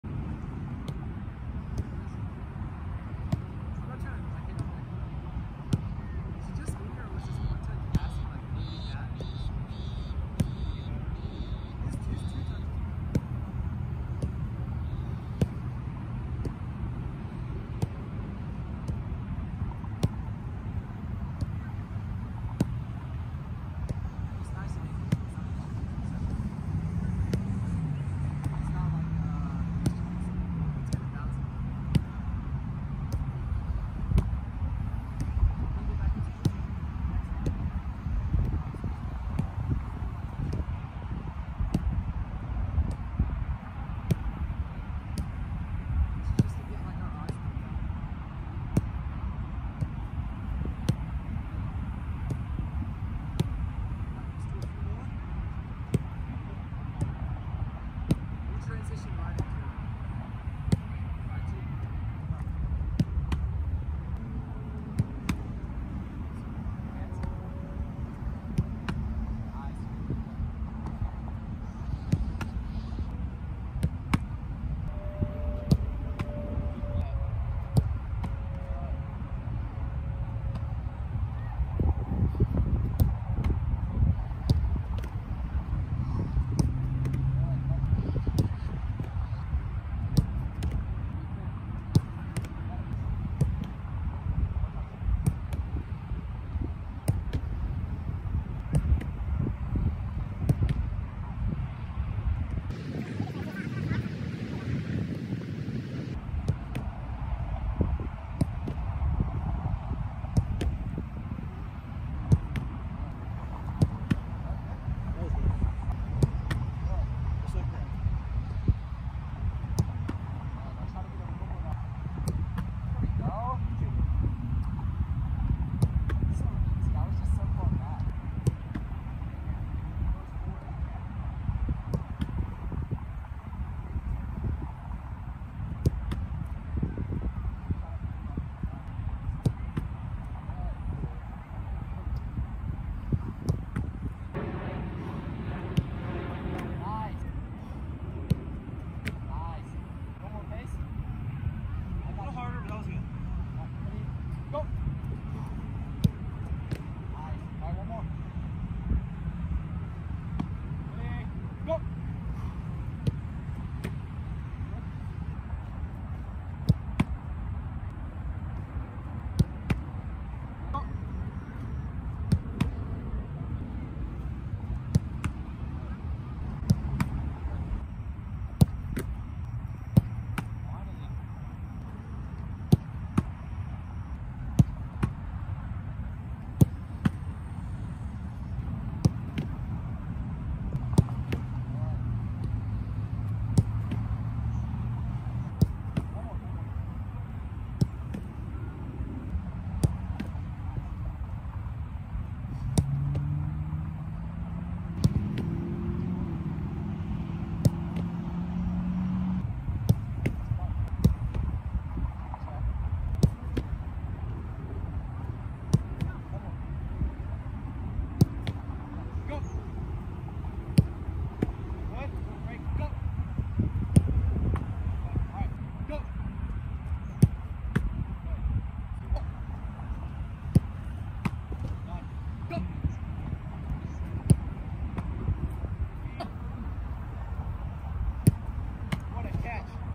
Training 8/18/21 - Handling and Diving (Timestamps Included)
Timestamps:
0:00 Light warmup passing
1:02 Warmup volleys
1:14 Warmup kicks to the hands
1:24 Warmup forward dives
1:32 Left-side ground ball control
1:46 Right-side ground ball control
1:53 Left-side knee dives
2:08 Right-side knee dives
2:24 Dipping volleys
2:45 Regular-paced volleys
2:57 Regular-paced kicks to the hands
3:08 Right-side ground dives
3:20 Left-side ground dives
3:28 Turn around/get up then shot

Grant Jackson '22
St. Mark's School of Texas
2021/22 Team: Dallas Texans U19 ECNL RL

Zak Downes
Goalkeeper Coach

Zeph Badii
Dallas Texans U19 ECNL RL Head Coach

Cory Martin
St. Mark’s Varsity Soccer Head Coach